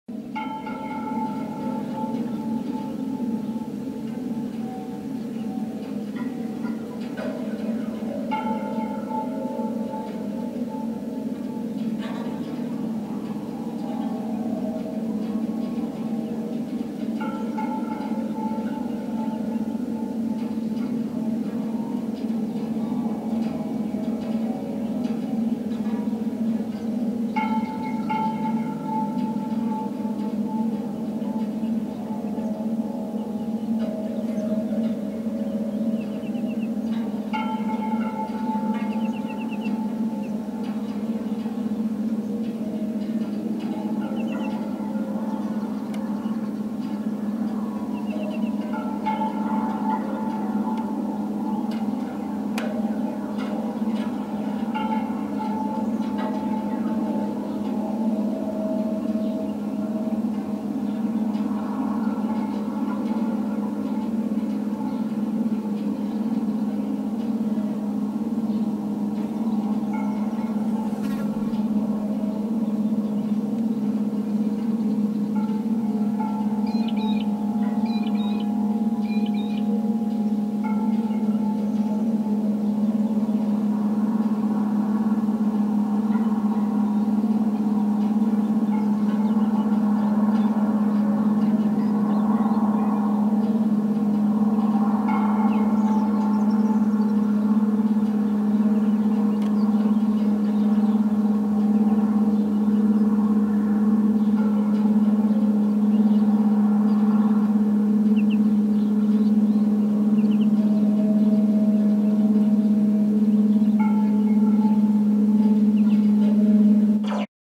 Aeolian harp on a quiet morning
for more about this large-scale aeolian harp.

Bit of a tease as the wires don't start singing until the end.